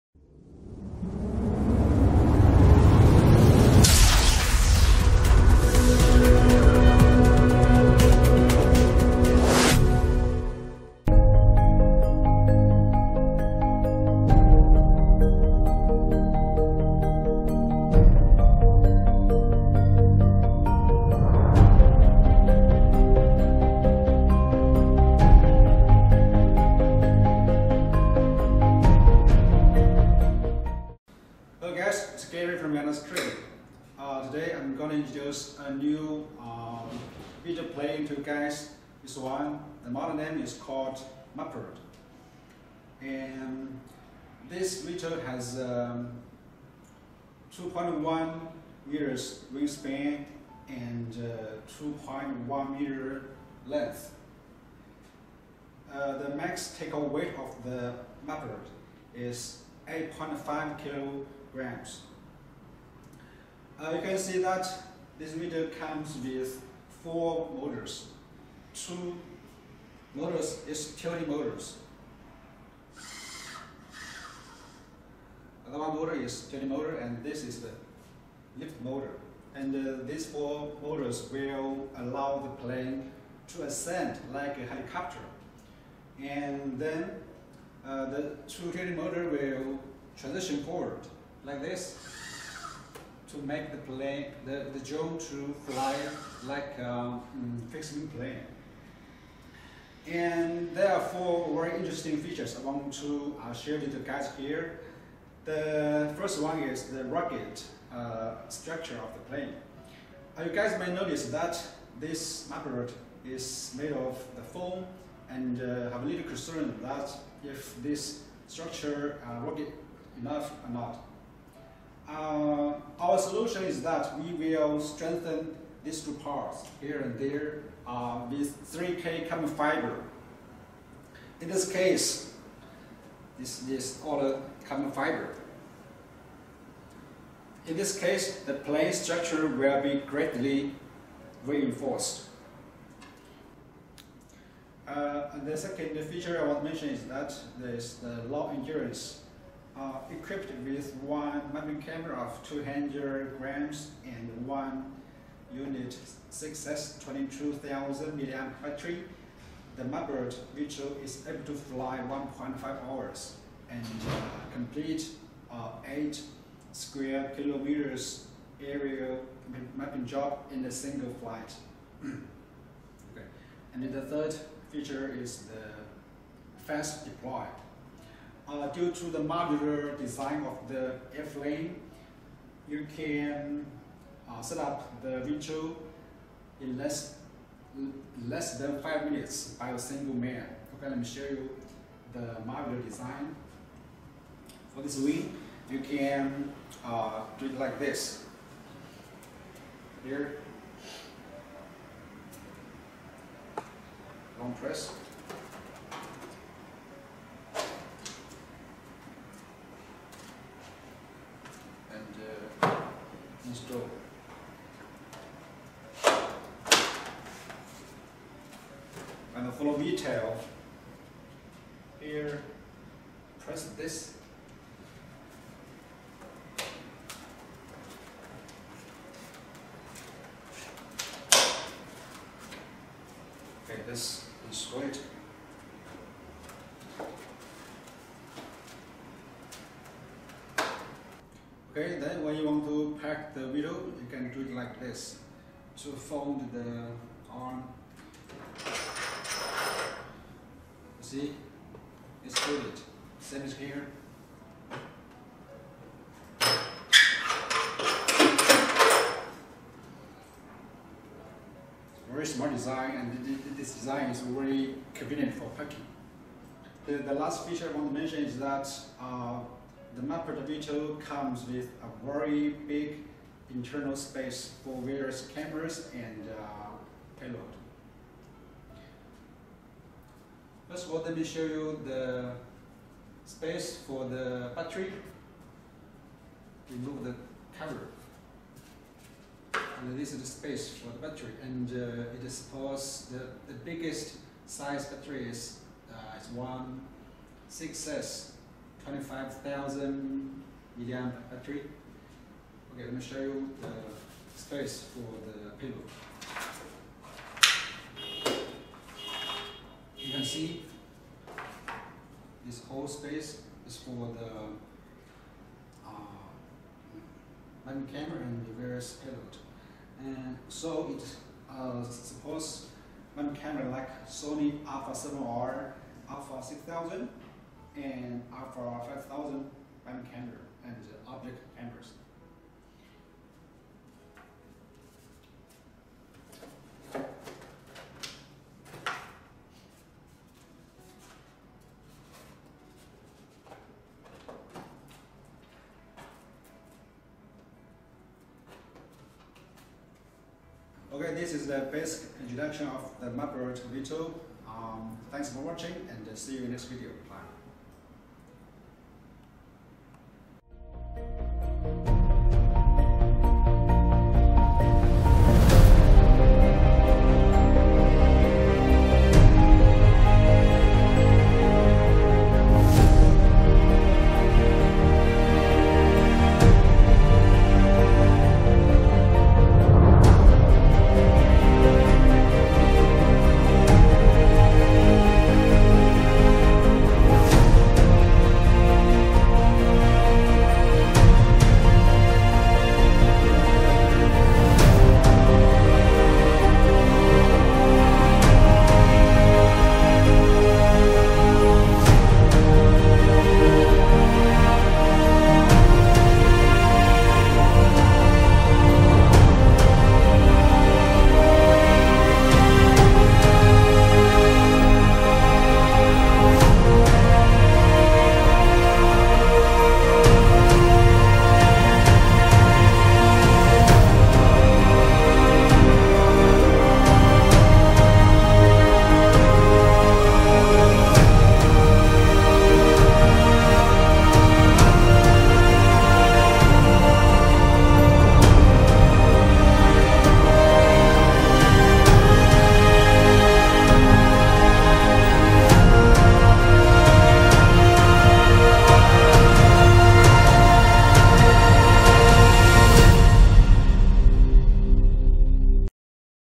Introducing Mapird: professional VTOL fixed-wing for mapping and survey
YANGDA Mapird is a professional VTOL(vertical take-off and landing) fixed-wing drone for mapping, aerial survey, and inspection mission. It comes with two tilting motors and two lift motors, which will allow the mapping drone to ascend like a helicopter. For the airplane survey mission, the front two tilting rotors will transition to forward mode and make the drone fly like a fixed-wing plane.

Rugged airframe
We reinforce the two wings of Mapird VTOL drone using 3K carbon fiber. Therefore, the frame structure is greatly strengthened.

Fast deploy
Due to its modular airframe design, Mapird mapping VTOL can be set up in less than 5 minutes by a single man.

Long endurance
Equipped with one 200 grams mapping camera, one unit 6S 22000mAh battery, the Mapird survey drone can fly up to 1.5 hours and complete an 8 square kilometer area mapping job in a single flight.

Big inner space for payload
Mapird mapping drone comes with a big internal space to accommodate various mapping cameras and payload, like SONY A7R/A6000/A5100 camera and oblique camera.

No piloting skills needed
Mapird can conduct fully autonomous mapping and survey missions without human interaction.